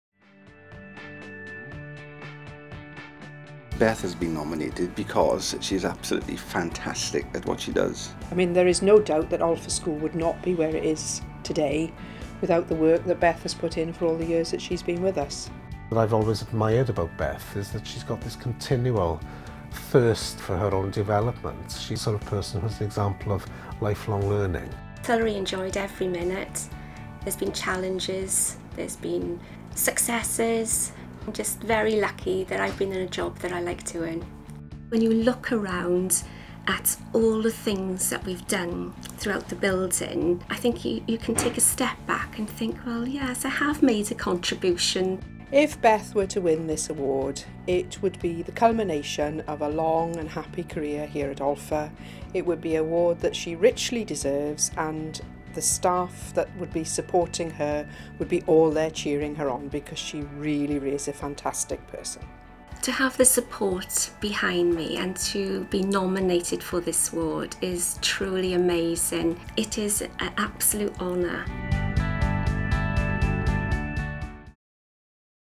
Enillydd/Winner: Bethan Baglow Ysgol yr Olchfa School
Enillydd y Wobr ar gyfer Rheolwr Busnes Ysgol/Bwrsar
Winner of the Award for School Business Manager/Bursar